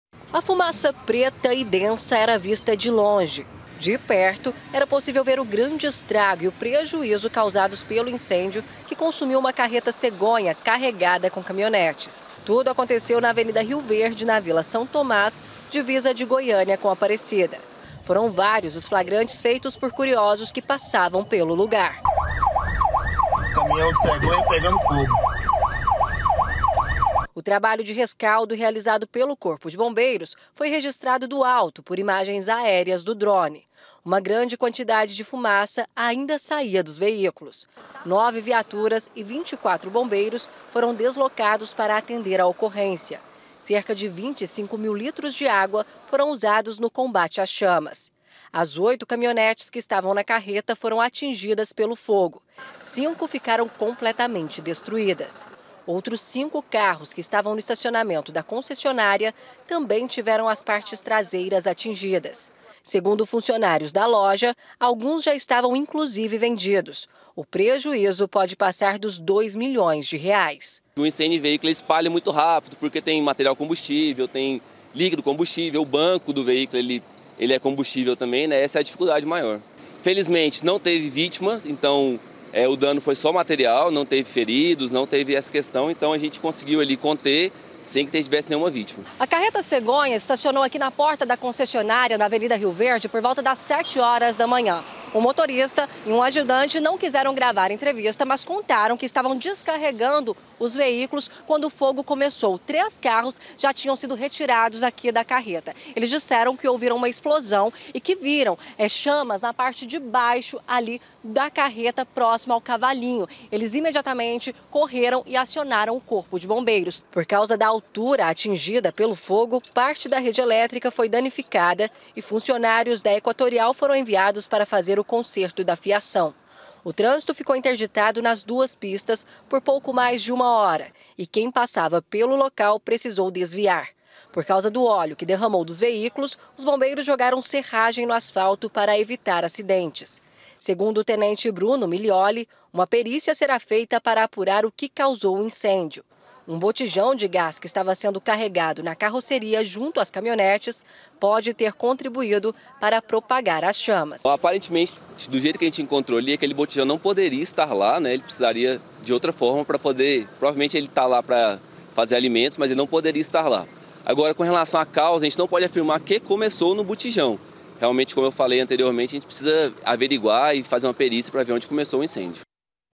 INCÊNDIO SERÁ INVESTIGADO: CAMINHÃO CEGONHA PEGA FOGO E DESTRÓI VEÍCULOS
UM CAMINHÃO CEGONHA CARREGADO DE CAMINHONETES PEGOU FOGO HOJE DE MANHÃ.